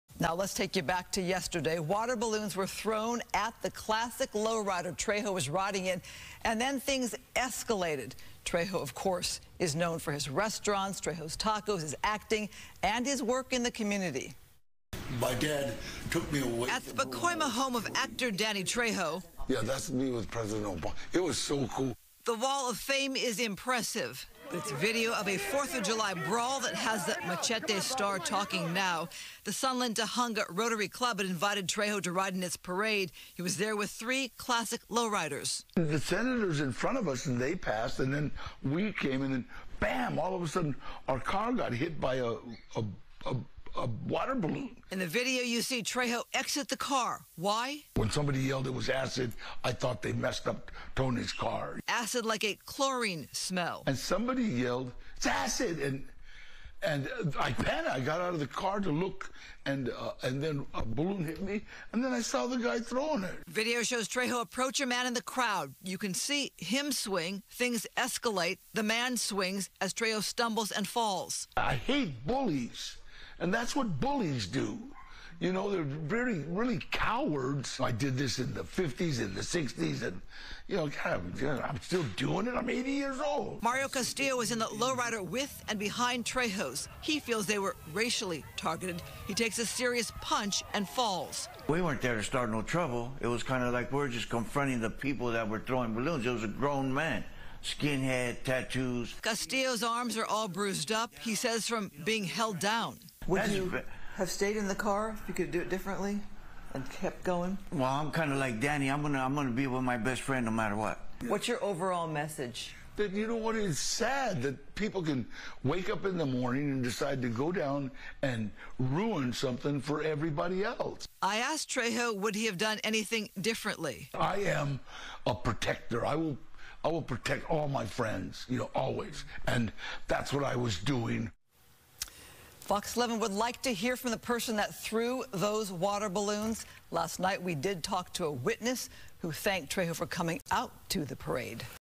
Danny Trejo tells his side of the story after Los Angeles 4th of July parade fight
Actor Danny Trejo is telling his side of the story after the "Machete" actor and his friend got into a fight with a crowd at an Independence Day parade in Los Angeles.